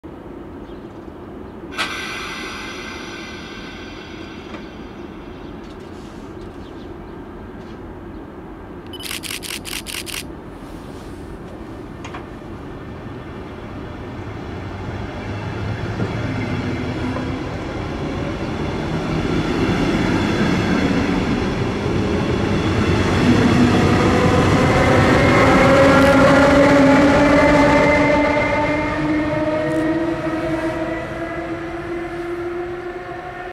CFL Dosto 016 en CFL 3017 vertrekken in Mamer
CFL Dosto 016 en CFL 3017 vertrekken in Mamer als RB trein naar Luxemburg (stad)

Dinsdag 23 april 2024